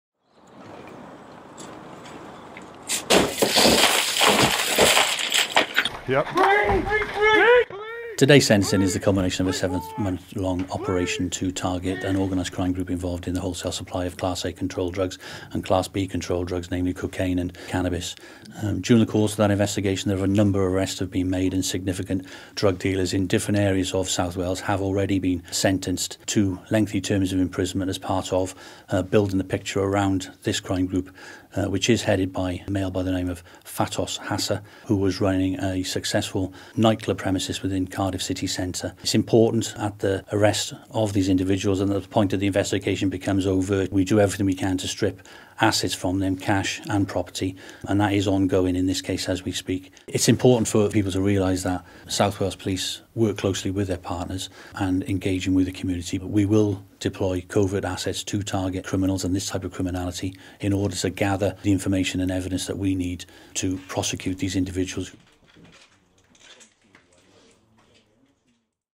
Drugs Gang Jailed - Operation Bounty
Five men have been jailed for their part in bringing large quantities of cocaine to the streets of South Wales.

The convictions are the culmination of Operation Bounty, a seven month police operation targeting the supply of controlled drugs into South Wales by an organised crime gang.

A number of simultaneous raids took place in March and April, 2015 involving around 50 officers, with the five men arrested at various addresses in Cardiff and Newport.

Three kilograms of cocaine and seven kilograms of cannabis, worth an estimated £1m, were seized during the course of the operation.

The head of the group was 31-year-old Fatos Hasa, who at the time of the offences was the owner of popular Cardiff cocktail bar Lab 22.

During the arrest phase of the operation, police raided his home in Coed Y Gores, Llanedeyrn, Cardiff where he was arrested.

As part of Operation Bounty police also seized around £147,000 from Mr Hasa's bank account and further work is going on to recover additional assets from the gang members.

Hasa, who is originally from Albania, was sentenced today at Newport Crown Court after pleading guilty to conspiracy to supply cocaine between August 2012 and April 2015 and conspiracy to supply cannabis between February 2014 and April 2015.

Jailing Hasa for eight years, Judge Philip Richards told him: "This was a significant drugs operation that aimed to distribute drugs to the population of this city (Cardiff). This was done over a long period, the evidence overwhelmingly pointed to you as the leader. You benefited substantially and you are a greedy man.
"You callously carried out your drug activities which has undoubtedly caused misery."

The other members of the gang received the following sentences, after all entering guilty pleas.

ï Lorenc Hasa, 29, from Coed Y Gores, Llanedeyrn, Cardiff ñ 68 months for conspiracy to supply cocaine.
ï Naldi Metaj, 23, of no fixed abode - 68 months for conspiracy to supply cocaine.
ï Sharif Omar, 34, from Carlisle Street, Splott, Cardiff ñ 40 months for conspiracy to supply cocaine.
ï Dardan Merdhoqi, 24, from Edison Ridge, Newport ñ 34 weeks for conspiracy to supply cannabis.

Detective Inspector Tudor Thomas from the Force Intelligence and Organised Crime Unit (FIOCU) at South Wales Police said:

Operation Bounty was launched as a result of information that we received from the community and today's sentences are the culmination of a seven month operation
The arrest and conviction of this crime group shows that South Wales Police will continue to pursue those who traffic drugs into our communities. The success of this investigation was as a direct result of South Wales Police personnel working alongside our communities to target such criminality.

He added: The sentences passed today give a clear and unequivocal message to those involved in the supply of controlled drugs and organised crime in South Wales. Our aim is to ensure that our communities remain safe and free from such criminality and we will continue to relentlessly pursue those involved in organised crime and bring them to justice.î

Catrin Evans, Head of the Complex Casework Unit for the Crown Prosecution Service Wales, said: The sentencing of this criminal gang represents another successful outcome for the highly professional investigation and prosecution teams who work in partnership to protect communities throughout Wales.

Drugs bring misery to our communities and their presence encourages other types of criminality. Those who seek to deal drugs within our local communities should be in no doubt that we will pursue them to the fullest extent of the law and will also seek to use Proceeds of Crime legislation to ensure that they are not able to retain the profits from their illegal activity.